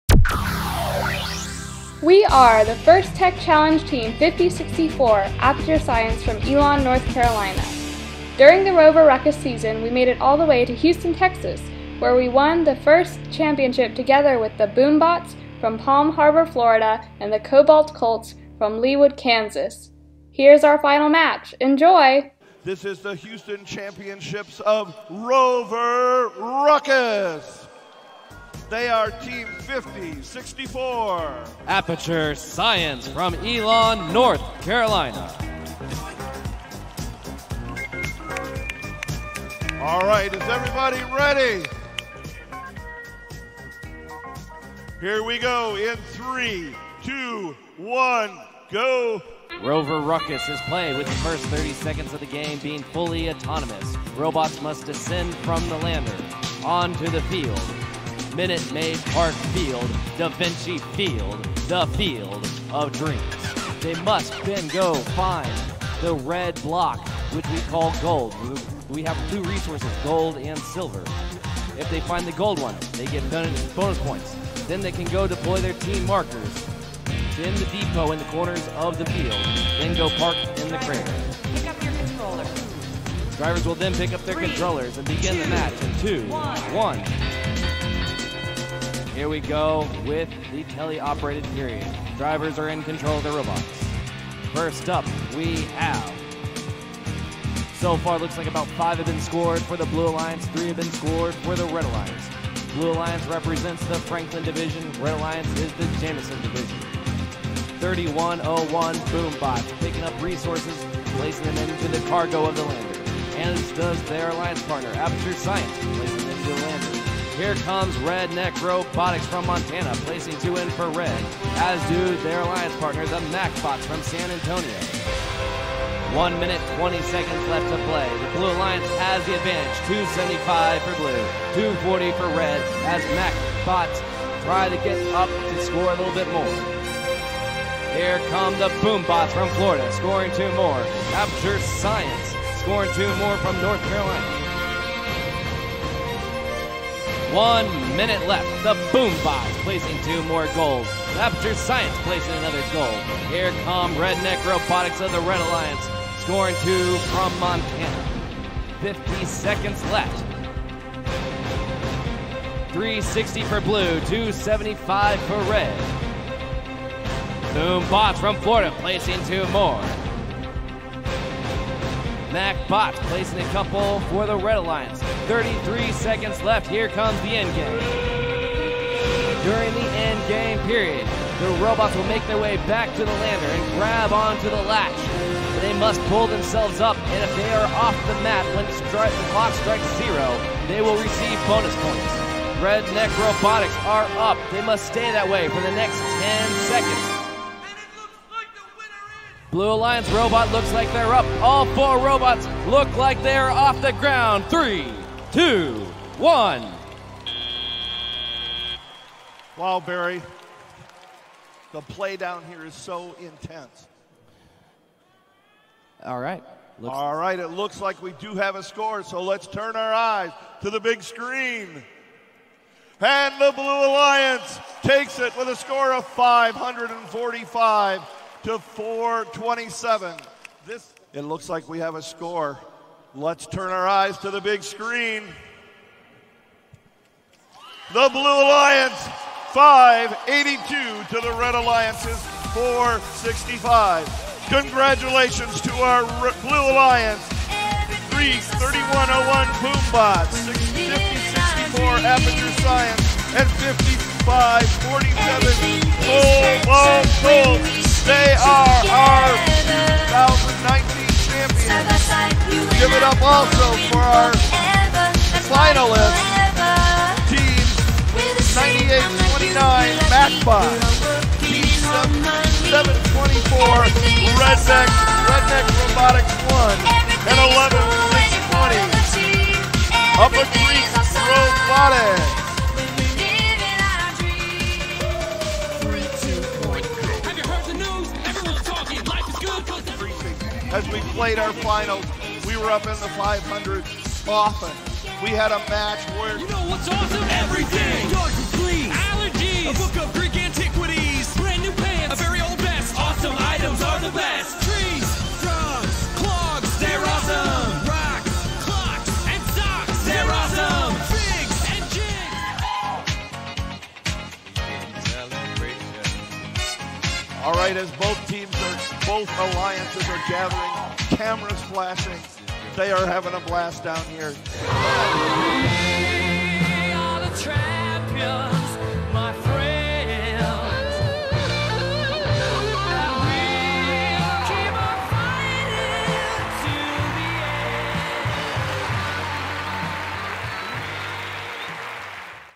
Aperture Science Finals at the 2019 FTC Championship in Houston
The Aperture Science robotics team (FTC-5064) made it all the way to the FIRST Championship in Houston in April of 2019. Together with our alliance captains, the Boom Bots (FTC-3101) from Palm Harbor, FL and the Cobalt Colts from Overland Park, KS, we won the FIRST Tech Challenge championship. This clip shows our final match and close-up footage of our robot.